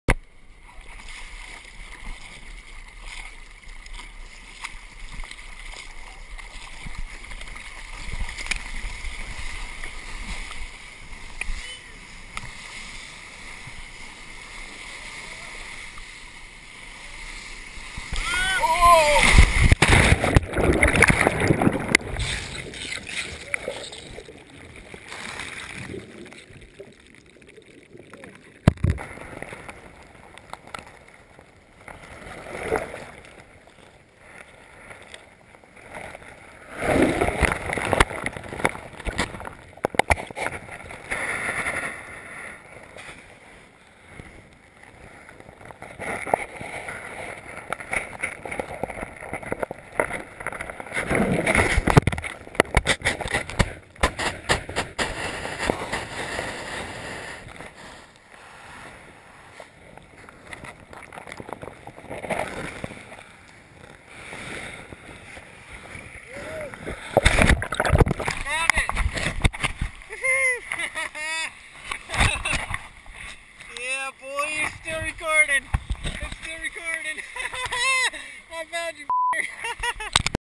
EPIC Surfing Rock Crash & GoPros Don't Float Lost and Found
After borrowing the GoPro Camera, my brother takes it surfing.  A little lesson for the rest, make sure the head strap is tightened before you decide to surf into a rock!  GoPro did great, after tumbling along the sea floor for 28 minutes, it was still rolling and recording, virtually undamaged when finally recovered!  Amazing! We learned the hard way that the GoPro HD Hero doesn't float!